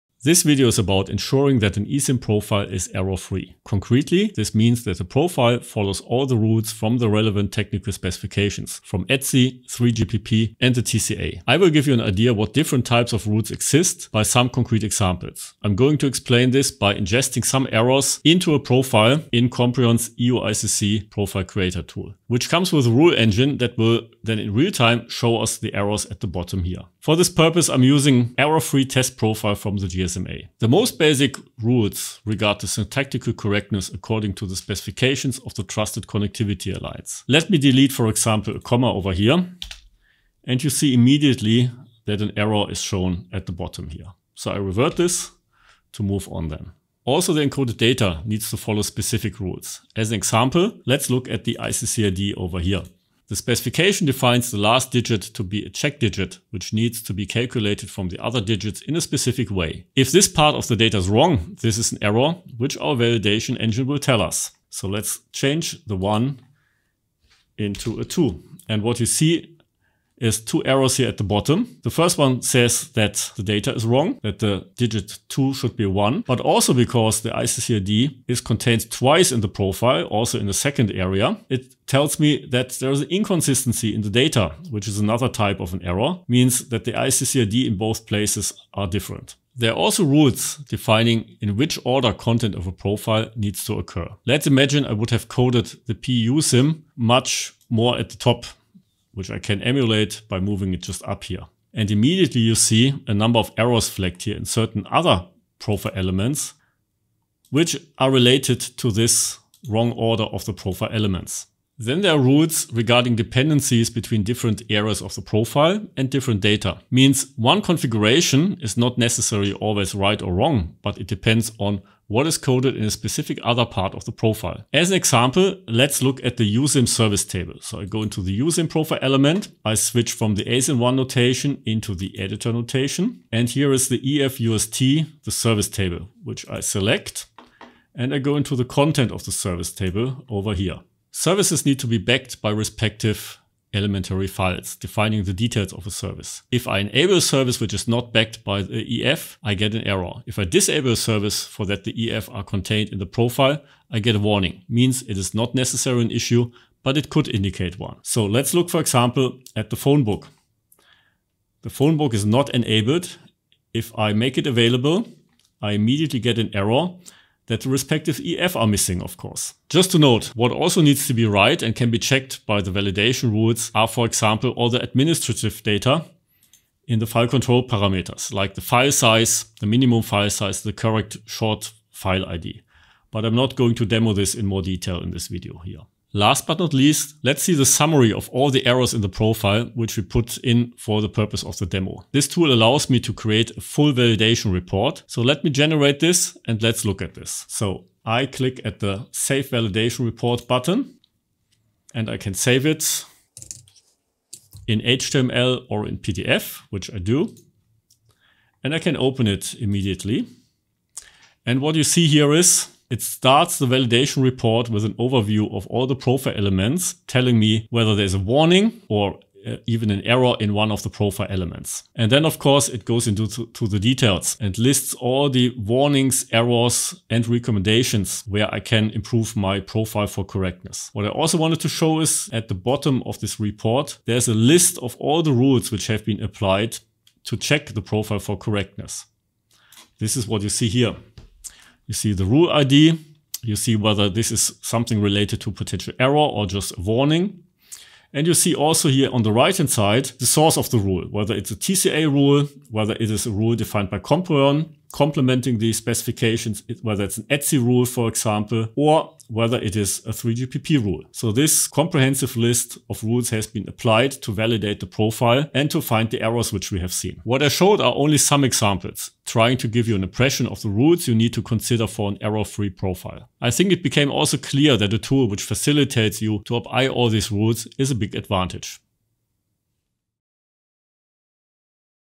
How To Make Sure an eSIM Profile is Error-Free
This video is about ensuring that an eSIM profile is error-free. Concretely, this means that the profile follows all the rules of the relevant technical specifications from ETSI, 3GPP and the TCA. Learn what different types of rules exist and how to deal with errors.